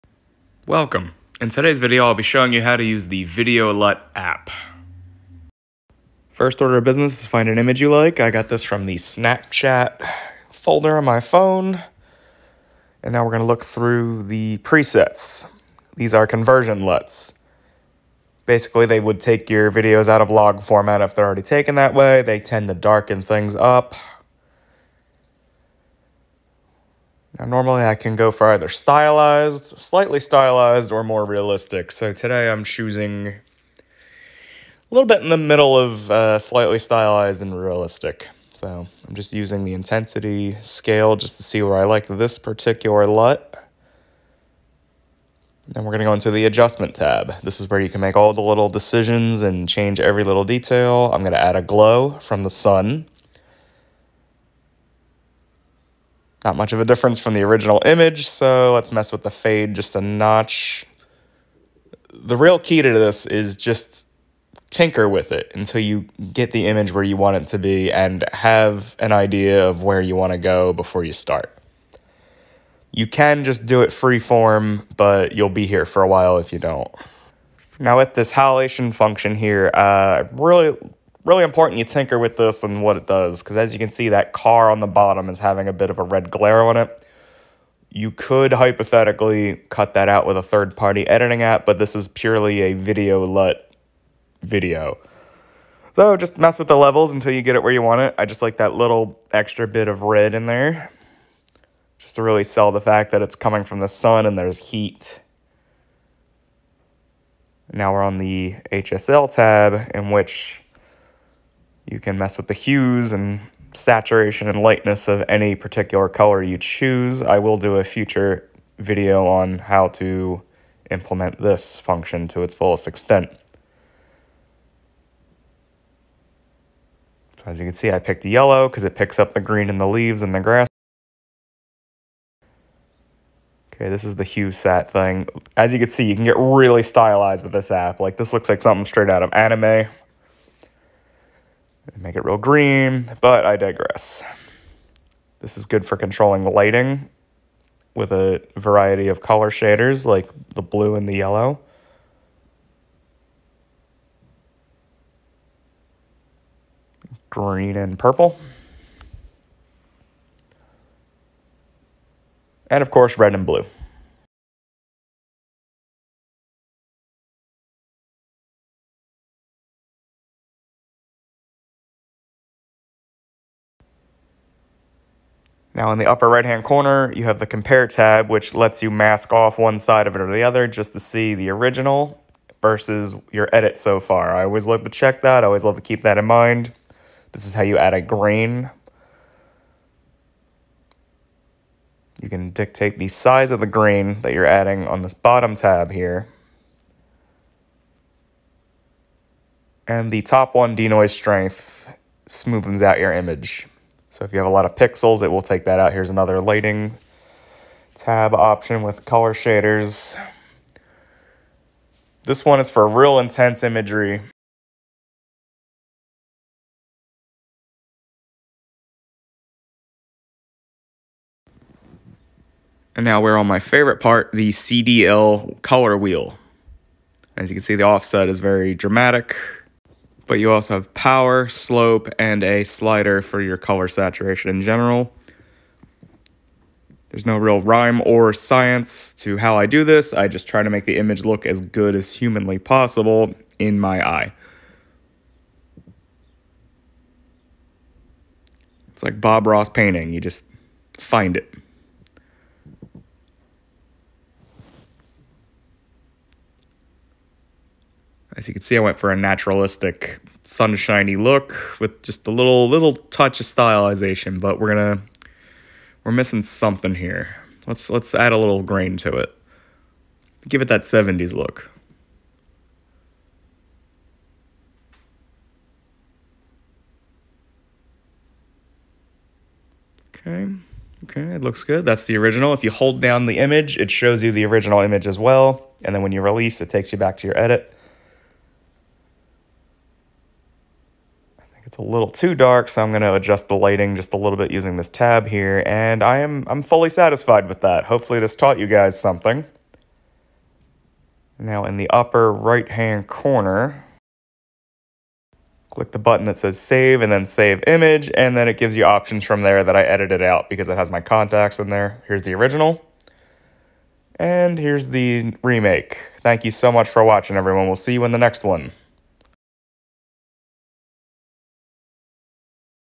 VideoLut App Tutorial - The Basics
In this video we will show you how to edit using our personal favorite color grading app: Videolut.